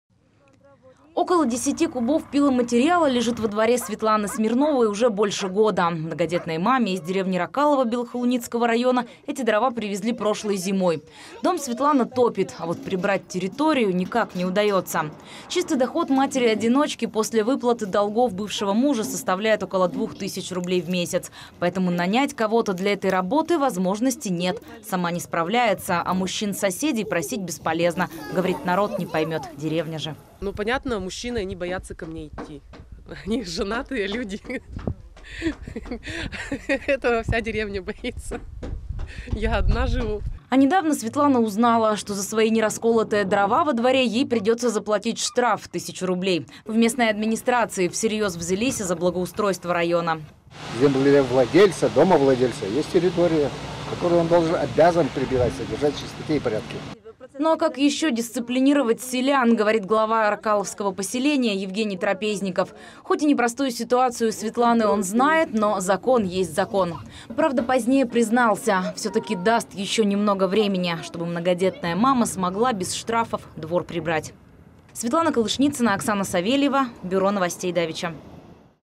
Сюжет о семье Смирновых из Ракалова
Телекомпания "СТС-9 канал": сюжет о семье Светланы Смирновой из деревни Ракалово. Благоустройство - это очень важно, а важно ли благополучие семьи?